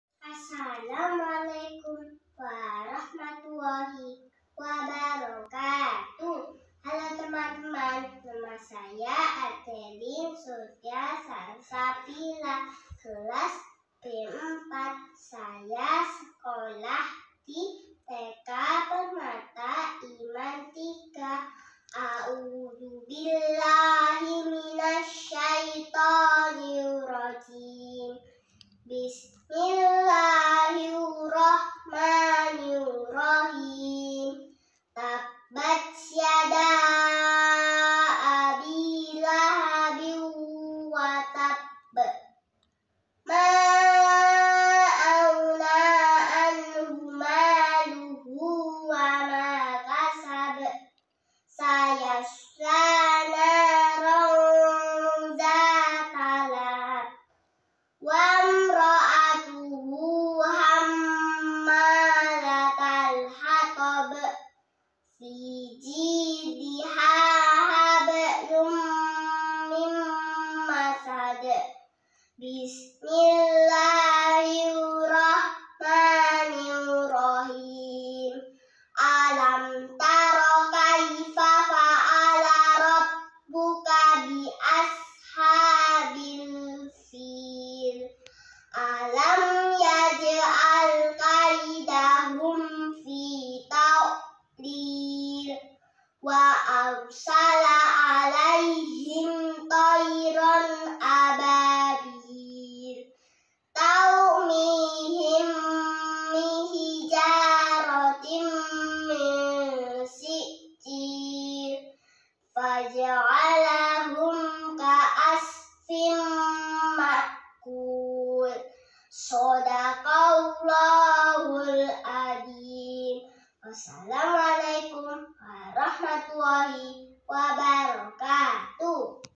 Adeline Surya Salsabila - TK Permata Iman 3 - LOMBA HAFALAN SURAT PENDEK - MILAD MI ISLAMIYAH KE-59
Dalam rangka memperingati hari Milad MI Islamiyah yang ke-59, sekolah mengadakan berbagai lomba yang menarik untuk diikuti.

Salah satunya yang ditampilkan dalam video ini merupakan hasil dari calon pemenang juara ke-3

Yang hasil keputusan akhir akan di hitung berdasarkan LIKE terbanyak.

Yuk kita dukung calon juara kita